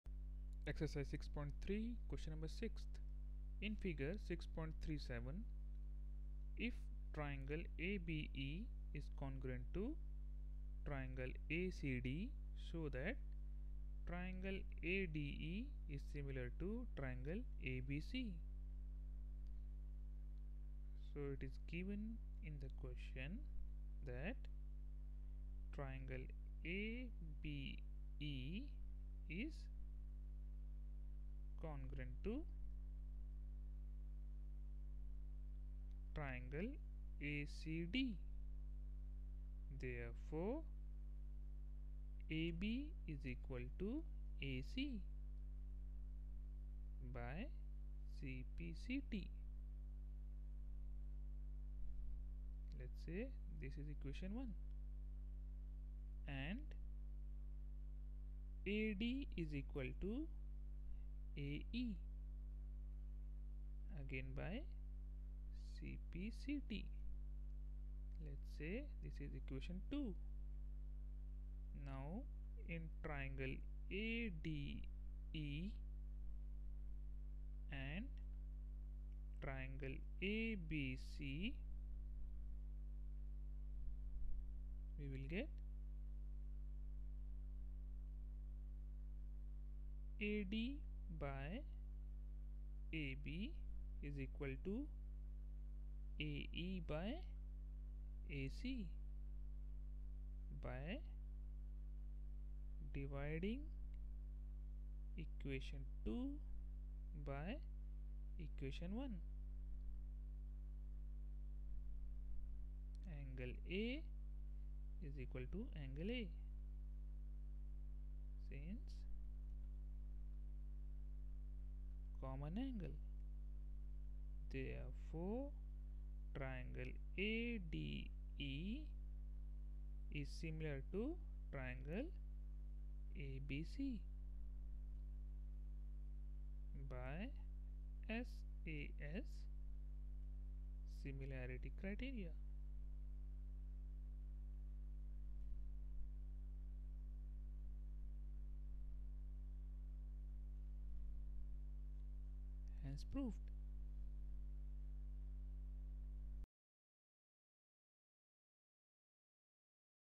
CBSE Class-10 Maths NCERT solution -Triangles- Exercise 6.3 - Problem 6
if ΔABE ≅ ΔACD, show that ΔADE ~ Δ ABC